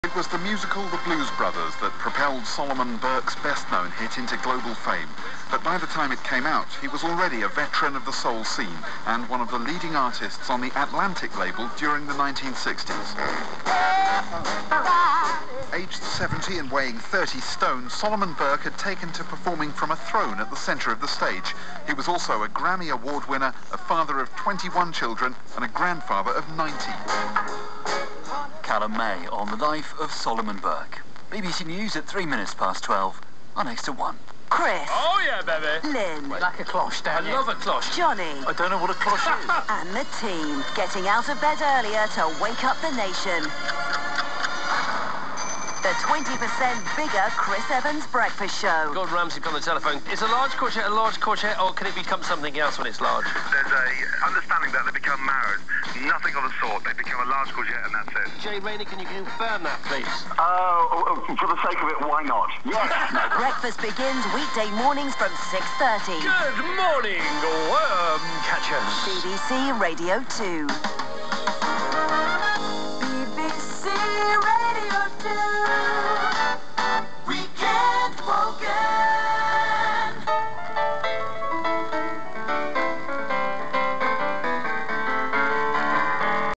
FM-DX (Tropo) 10.10.2010: BBC Radio 2, Holme Moss, G - 619 km
QTH: Geeste/Emsland (52°36'N/7°16'E), NW-Germany
RX: Lextronix E5

89,3 MHz (250 kW), Holme Moss, G
RDS: not received